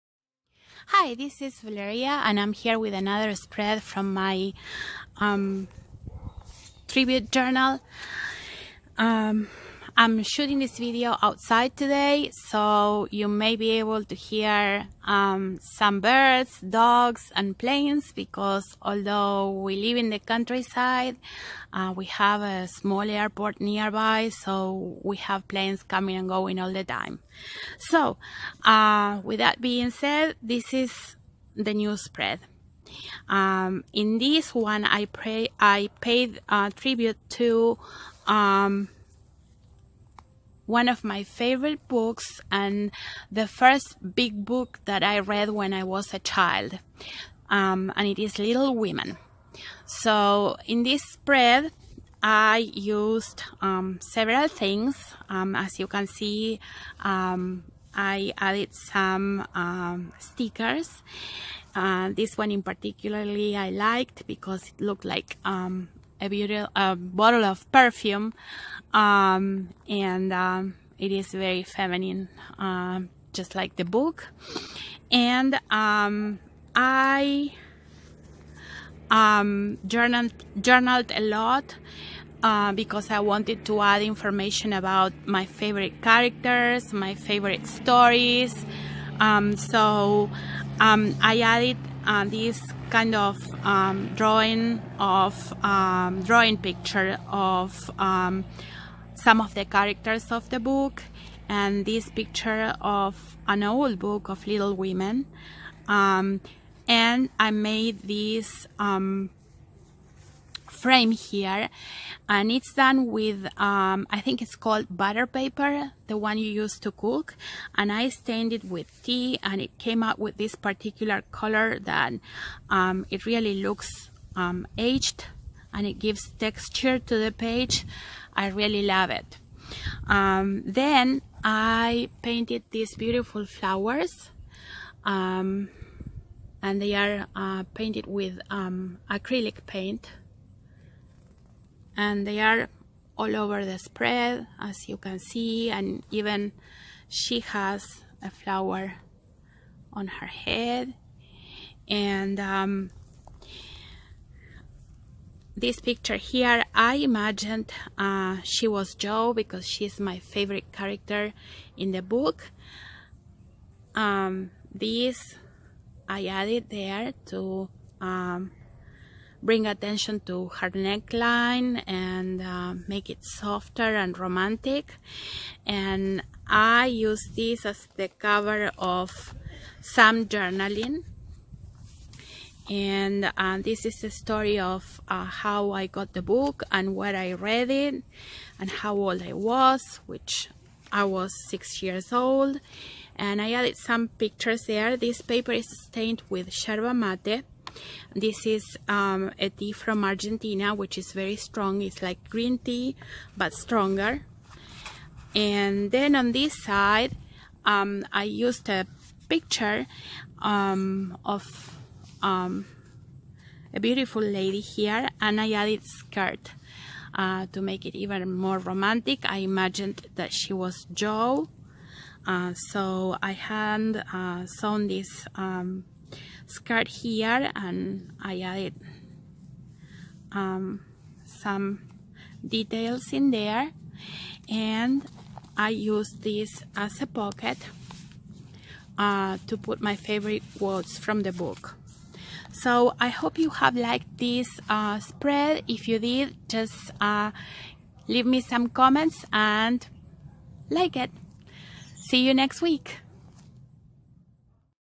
Little women spread in my Tribute Journal
This is a tribute to the first big book I read as a child. With hand painted flowers, hand sewn dress, hidden pockets, tea dyed paper and a lot of journaling, I tried to bring out all the femenine aspect of such a romantic era!